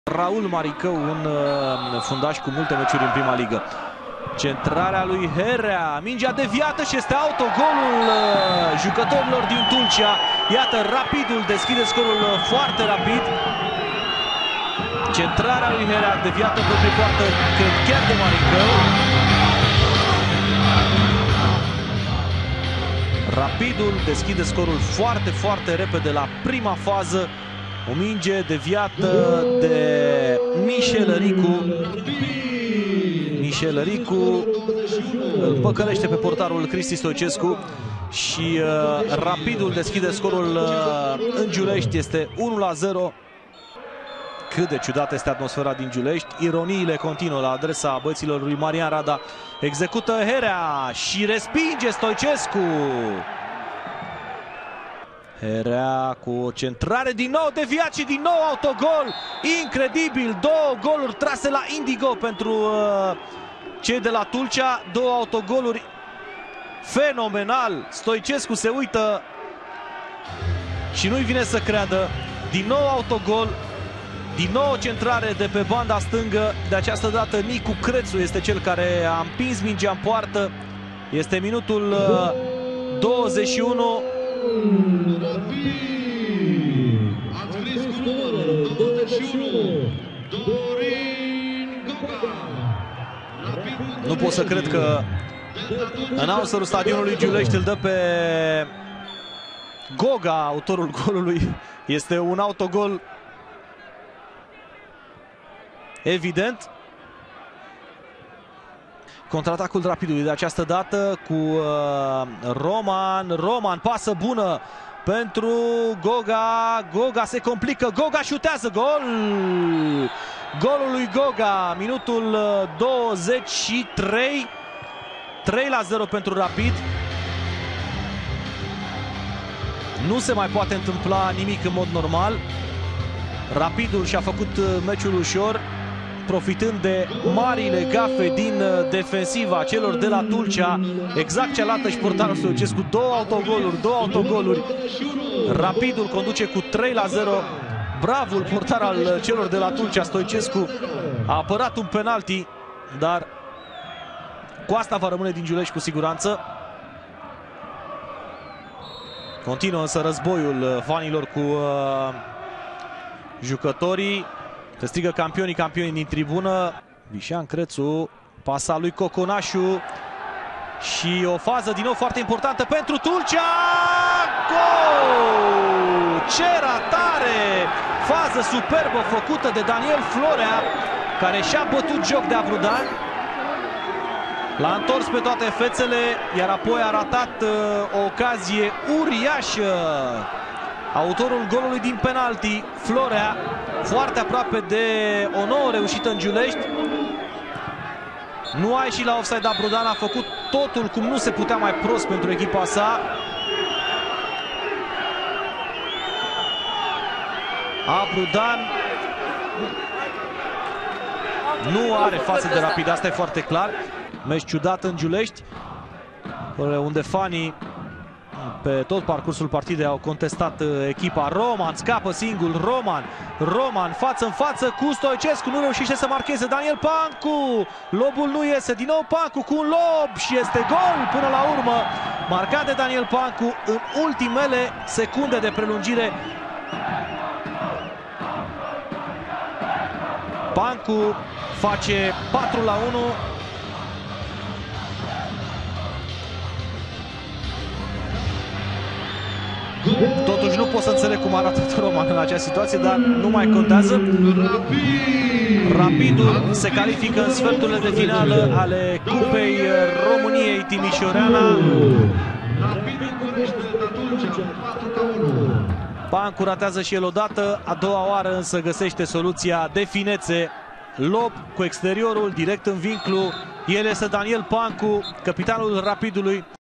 Cupa Romaniei|8-imi: FC RAPID Bucuresti-FC Delta Tulcea 4-1
Sursa:sport.ro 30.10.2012. Marcatori: Autogol (04', 22'), Goga (24'), Pancu (90+3')/Florea (40'-pen.).